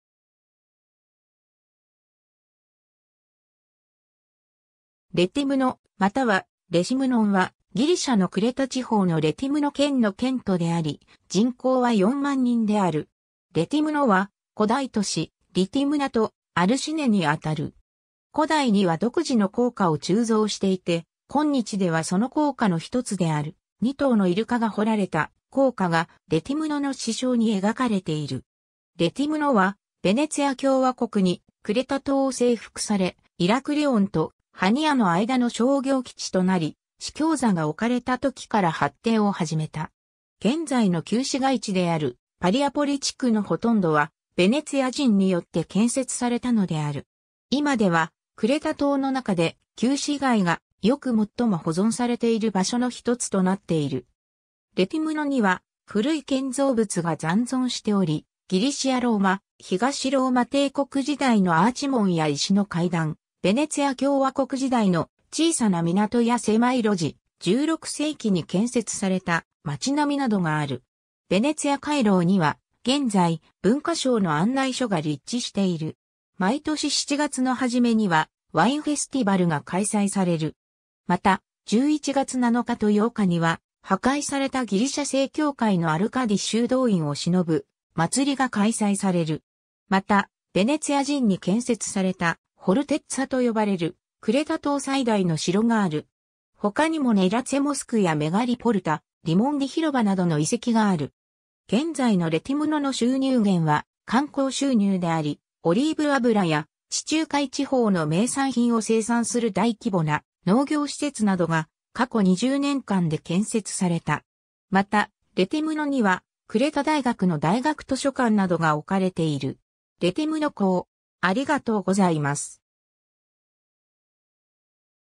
レティムノ
レティムノまたは、レシムノン（ギリシャ語:Ρέθυμνο/Rethymno）は、ギリシャのクレタ地方のレティムノ県の県都であり、人口は40,000人である。
レティムノは古代都市リティムナ(Rhithymna)とアルシネ(Arsinoe)にあたる。
古代には独自の硬貨を鋳造していて、今日ではその硬貨の一つである、2頭のイルカが彫られた硬貨が、レティムノの市章に描かれている。
 レティムノは、ヴェネツィア共和国にクレタ島を征服され、イラクリオンとハニアの間の商業基地となり、司教座が置かれた時から発展を始めた。
現在の旧市街地であるパリア・ポリ地区のほとんどは、ヴェネツィア人によって建設されたのである。
今ではクレタ島の中で、旧市街がよく最も保存されている場所の一つとなっている。
 レティムノには古い建造物が残存しており、ギリシア・ローマ、東ローマ帝国時代のアーチ門や石の階段、ヴェネツィア共和国時代の小さな港や狭い路地、16世紀に建設された街並みなどがある。
「ヴェネツィア回廊」には現在、文化省の案内所が立地している。
毎年7月の初めにはワイン・フェスティバルが開催される。
また、11月7日と8日には、破壊されたギリシャ正教会のアルカディ修道院を偲ぶ祭りが開催される。
また、ヴェネツィア人に建設された、フォルテッツァ(Fortezza)と呼ばれるクレタ島最大の城がある。
他にもネラツェ・モスク（現在は聖カトリーヌ教会）やメガリ・ポルタ（大きな門）、リモンディ広場などの遺跡がある。
 現在のレティムノの収入源は観光収入であり、オリーブ油や地中海地方の名産品を生産する大規模な農業施設などが過去20年間で建設された。
また、レティムノにはクレタ大学の大学図書館などが置かれている。
 レティムノ港